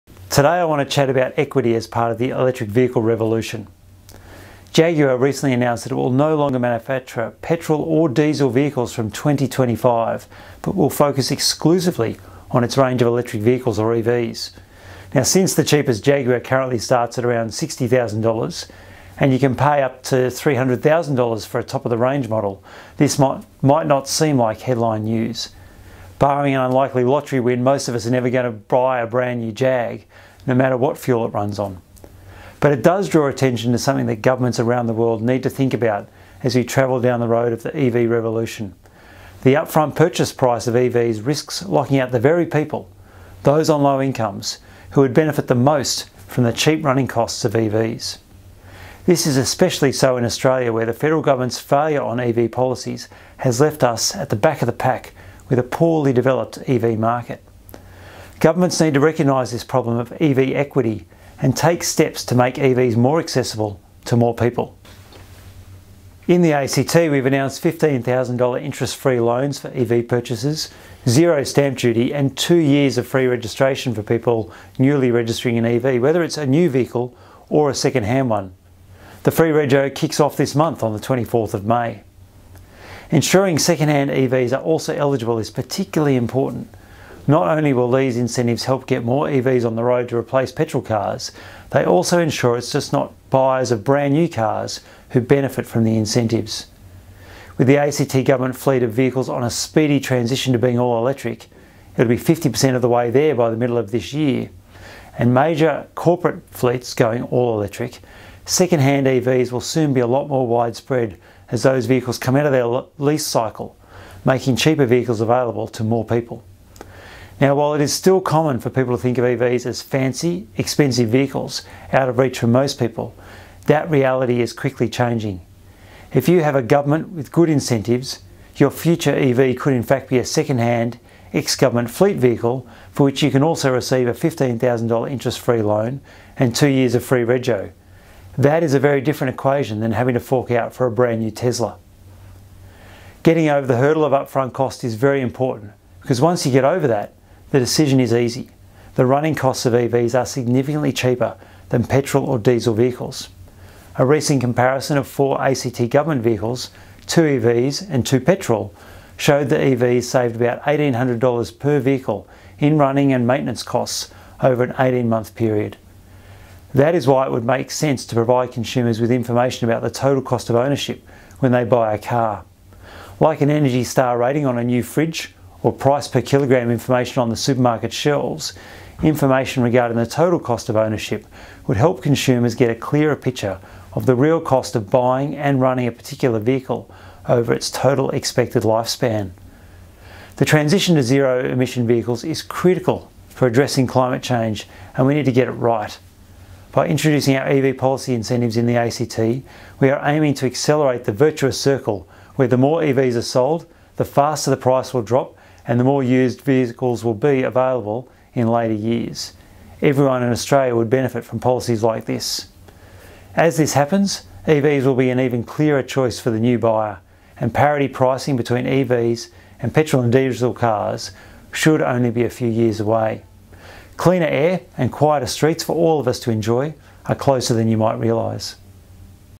Equity is a key to the electric vehicle revolution
By ACT Minister for Emissions Reduction, Shane Rattenbury.

Jaguar recently announced that it will no longer manufacture petrol or diesel vehicles from 2025 but will focus exclusively on its range of electric vehicles (EVs).

Since the cheapest Jaguar currently starts at around $60,000, and you can pay over $300,000 for a top-of-the-range model, this might not seem like headline news. Barring an unlikely lottery win, most of us are never going to buy a brand-new Jag no matter what fuel it runs on.

But it draws attention to something that governments around the world need to think about as we travel down the road of the EV revolution: The upfront purchase price of EVs risks locking out the very people – those on low incomes – who would benefit the most from the cheap running costs of EVs. This is especially so in Australia, where the Federal Government’s failure on EV policies has left us at the back of the pack, with a poorly developed EV market.

Governments need to recognise this problem of EV equity and take steps to make EVs more accessible to more people.

In the ACT, we’ve announced $15,000 interest-free loans for EV purchases, zero stamp duty, and two years of free registration for people newly registering an EV, whether it’s a new vehicle of second hand. The free rego offer starts this month, on 24 May.

Ensuring second-hand EVs are also eligible is particularly important. Not only will these incentives help get more EVs on the road to replace petrol cars, they also ensure it’s not just buyers of brand-new cars who benefit from the incentives.

With the ACT Government fleet of vehicles on a speedy transition to being all-electric (it will be 50% of the way there by the middle of this year), as well as other major corporate fleets going all-electric, second-hand EVs will soon be a lot more widespread, making cheaper vehicles available to more people.

While it is still common for people to think of EVs as fancy, expensive vehicles out of reach for most people, that reality is quickly changing. If you have a government with good incentives, your future EV could in fact be a second-hand, ex-government fleet vehicle, for which you can also receive a $15,000 interest free loan, and two years of free rego. That is a very different price equation than having to fork out for a new Tesla.

Getting over the hurdle of the upfront cost is very important, because once you get over that, the decision is easy - the running costs of EVs are significantly cheaper than petrol or diesel vehicles. A recent comparison of four ACT Government vehicles – two EVs, two petrol – showed the EVs saved about $1800 per vehicle in running and maintenance costs over an 18-month period.

That is why it would make sense to provide consumers with information about the total cost of ownership when they buy a car. Like an energy star rating on a new fridge, or price-per-kilogram information on supermarket shelves, information regarding the total cost of ownership would help consumers get a clearer picture of the real cost of buying and running a particular vehicle over its total expected lifespan.

The transition to zero-emissions vehicles is critical for addressing climate change and we need to get it right. By introducing our EV policy incentives in the ACT, we’re aiming to accelerate the virtuous circle where the more EVs are sold, the faster the price will drop and the more used vehicles will be available in later years. Everyone in Australia would benefit from policies like these.

As this happens, EVs will be an even clearer choice for the new buyer, and parity pricing between EVs and petrol and diesel cars should be only a few years away.

Cleaner air and quieter streets for all of us to enjoy is closer than you might realise.